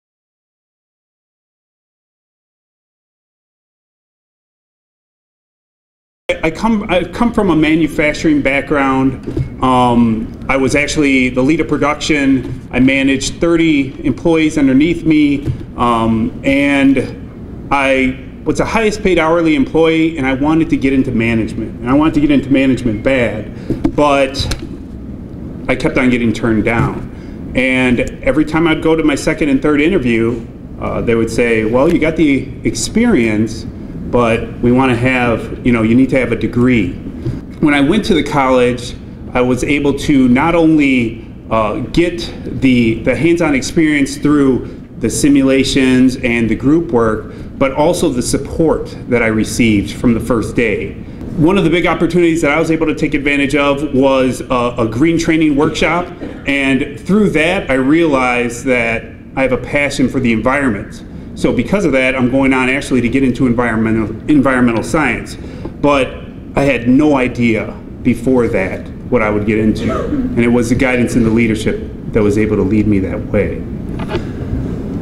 Blackhawk Technical College Ambassador
John Peralta, 2013 WTCS State Ambassador for Blackhawk Technical College discusses his time at BTC and how it helped him discover his passions.